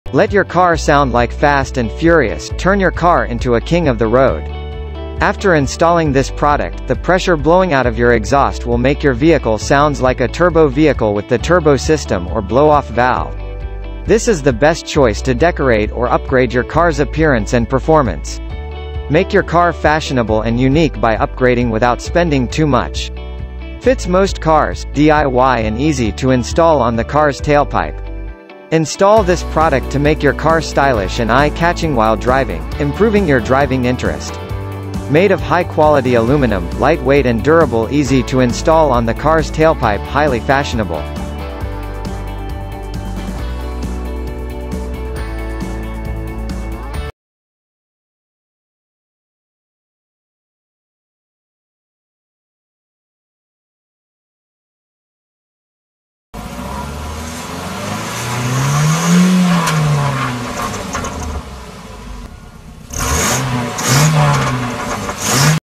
Multi Purpose Car Turbo Whistle Review 2020 - Does It Work?
Multi Purpose Car Turbo Whistle is the best choice to decorate or upgrade your car's appearance and performance. Make your car fashionable and unique by upgrading without spending too much.

Car Turbo Whistle Fits most cars, DIY and easy to install on the car's tailpipe. Install this product to make your car stylish and eye-catching while driving; improving your driving interest.

After installed this product, the pressure blowing out of your exhaust will make your vehicle sounds like a turbo vehicle with the turbo system or blow off valve.
Made of high quality aluminum, light weight and durable.
Easy to install on the car's tailpipe.